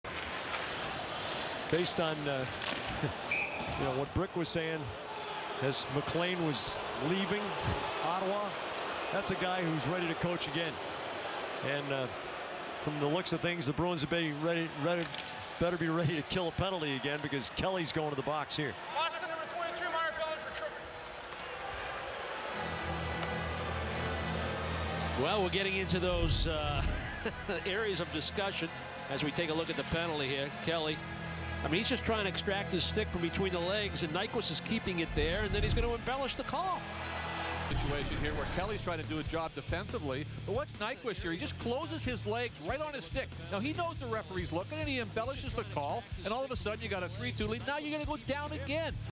Gustav Nyquist fined $2000 for diving on Chris Kelly
Gustav Nyquist was fined $2000 by the NHL for this dive, clutching the stick of Chris Kelly between his legs and falling to the ice, in the Wings-Bruins game on December 29, 2014.